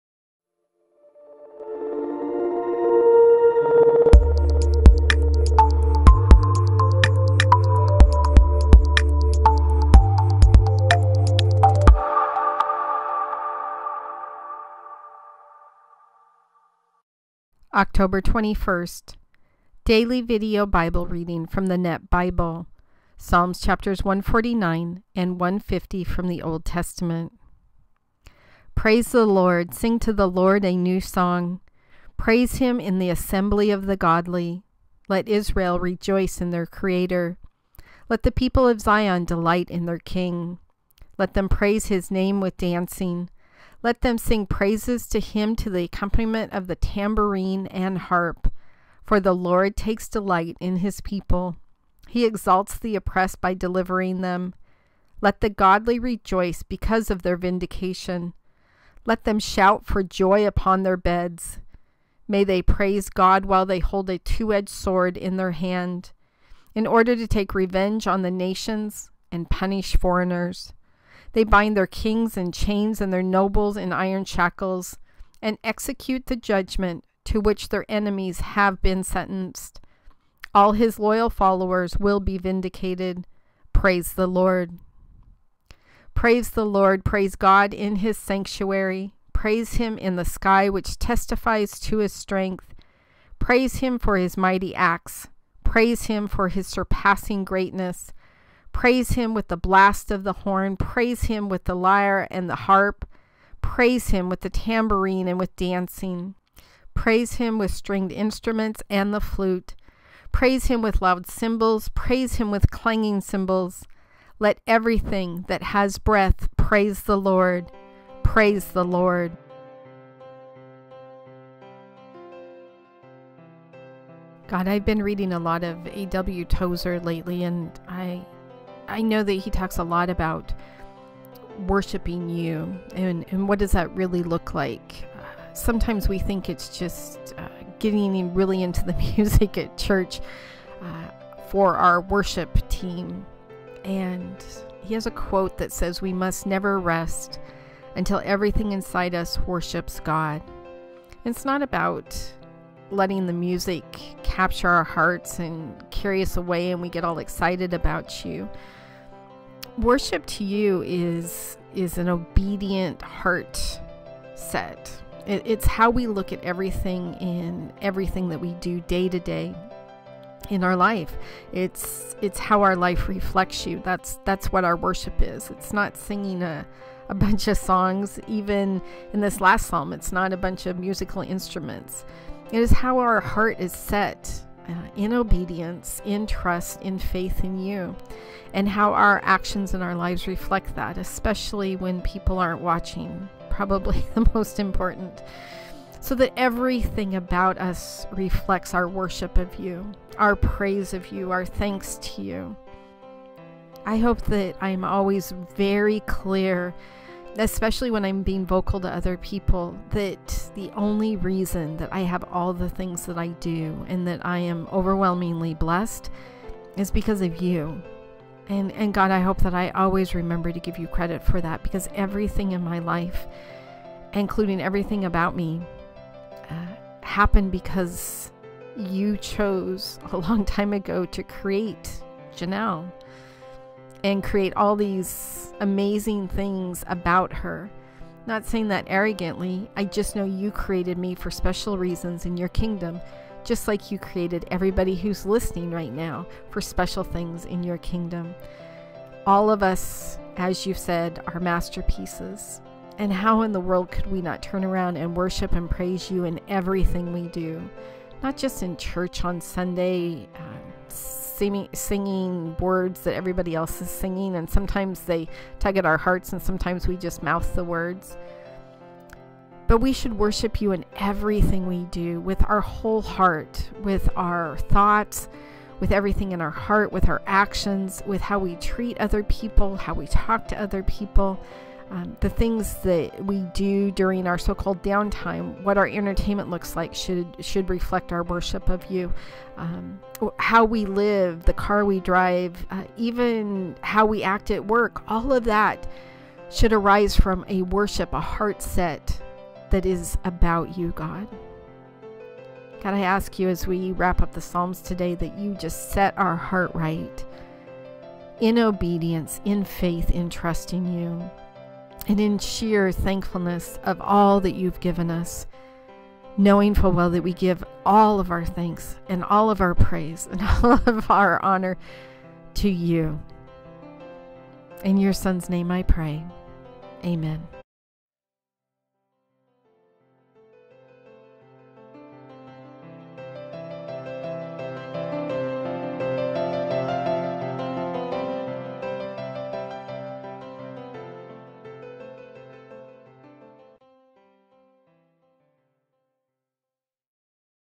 October 21 Psalms 149 and 150 Old Testament for the Daily Video Bible DVB
Psalms 149:1-9 - Psalm 149
149:1 Praise the LORD!
Sing to the LORD a new song!
Praise him in the assembly of the godly!
149:2 Let Israel rejoice in their Creator!
Let the people of Zion delight in their king!
149:3 Let them praise his name with dancing!
Let them sing praises to him to the accompaniment of the tambourine and harp!
149:4 For the LORD takes delight in his people;
he exalts the oppressed by delivering them.
149:5 Let the godly rejoice because of their vindication!
Let them shout for joy upon their beds!
149:6 May they praise God
while they hold a two-edged sword in their hand,
149:7 in order to take revenge on the nations,
and punish foreigners.
149:8 They bind their kings in chains,
and their nobles in iron shackles,
149:9 and execute the judgment to which their enemies have been sentenced.
All his loyal followers will be vindicated.